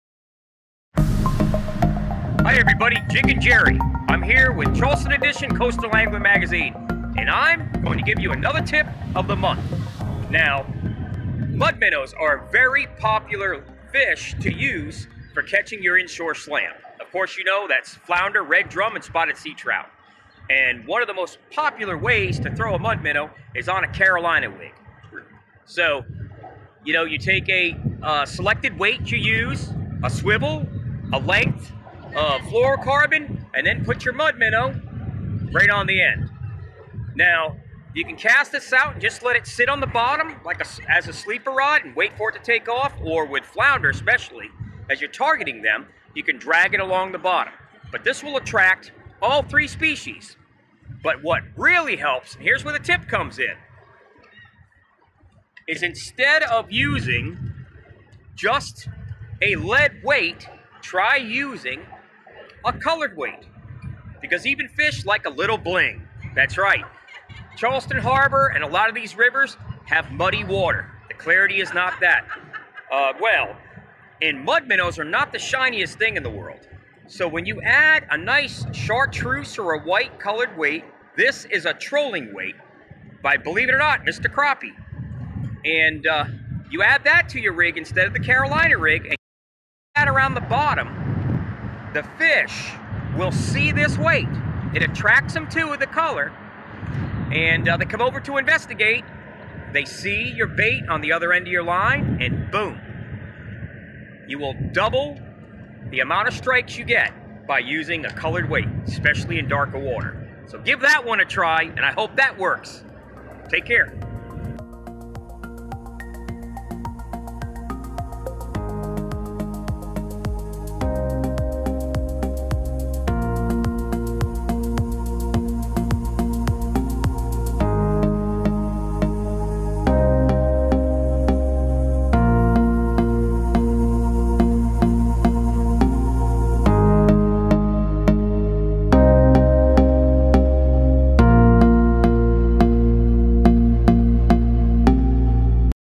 Double your catch with Mud Minnows on a Carolina rig - By Jiggin Jerry
Jiggin Jerry with this Tip of the Month shows you how to improve your strikes and catches using a simple Carolina rig and Mud Minnows, you'll be catching twice as many fish by using this technique!